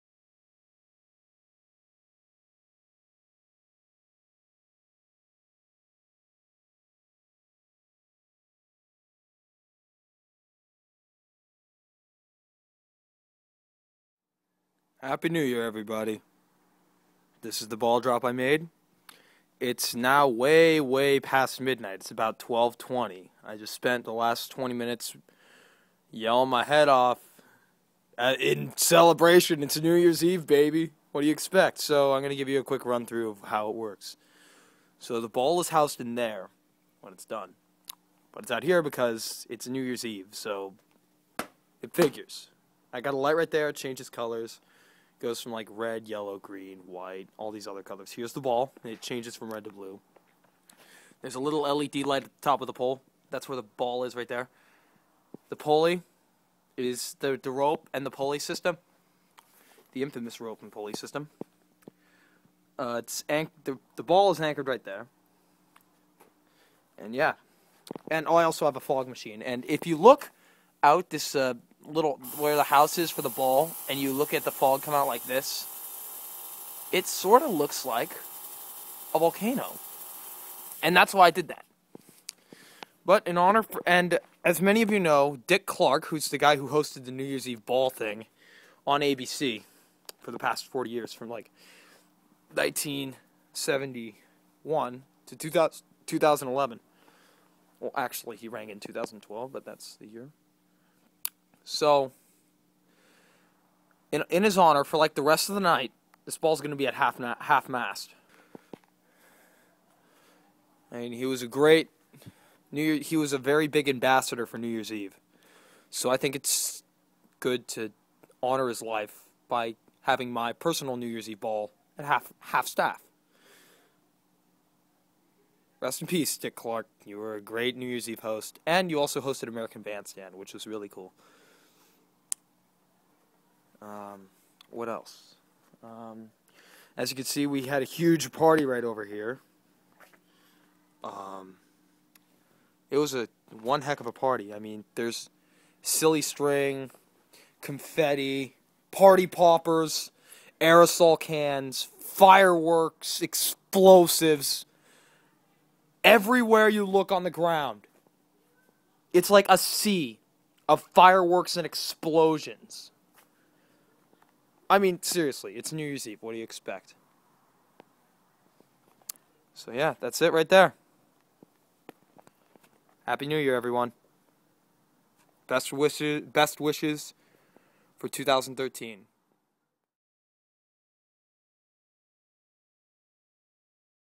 Behind The Scenes At The 3rd Annual Homemade New Year's Eve Ball Drop 2013
A short film on how the ball drop works, and how it is controlled.